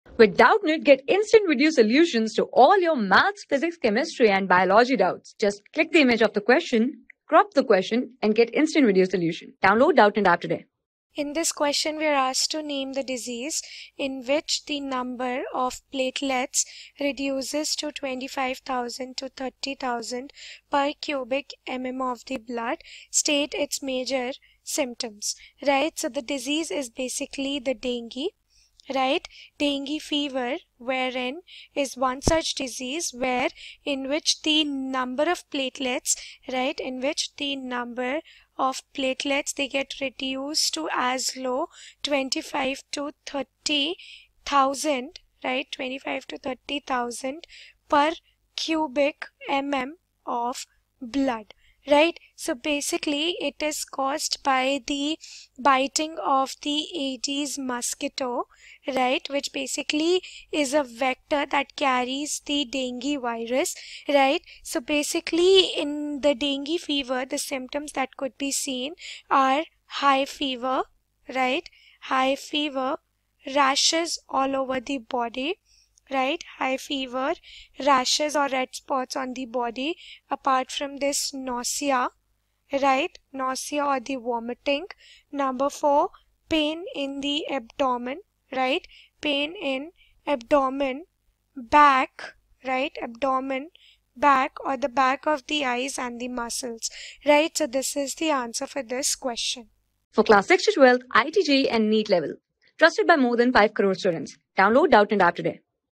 Name the disease in which the number of platelets reduces to 25,000-30,000 per cu m m of blood. ...
Name the disease in which the number of platelets reduces to 25,000-30,000 per cu m m of blood. State its major symptoms. .
Class: 6
Subject: BIOLOGY
Chapter: THE CIRCULATORY SYSTEM
Board:ICSE

👉 Have Any Query? Ask Us.

Our Telegram Pages:
class 6
6 class ka math syllabus